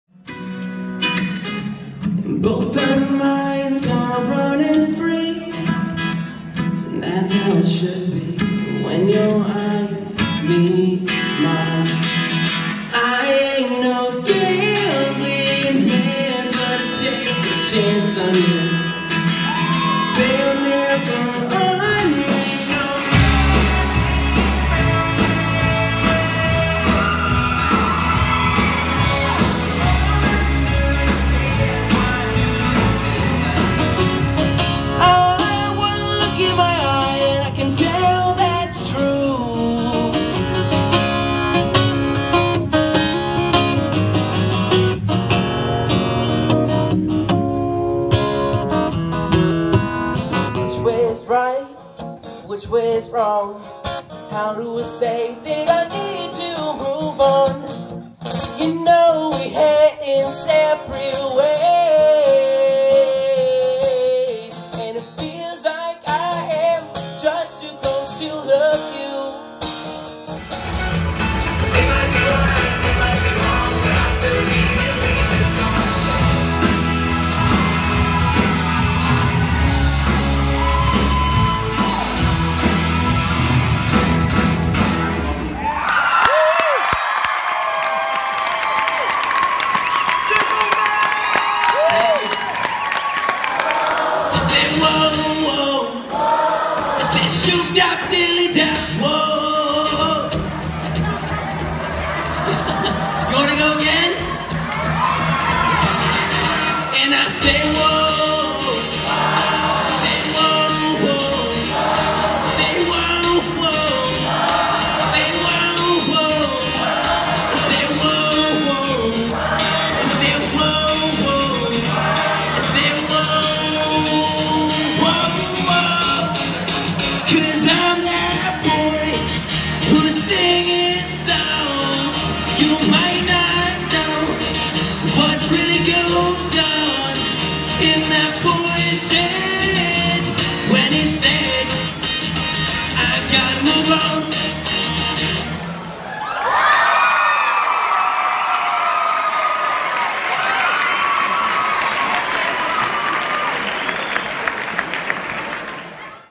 VIDEO - Sampling of Richard Performance
This is a short video aimed at giving you a quick sample of my performance style.  Some clips show solo and band performances as well as audience reaction at the end of the clip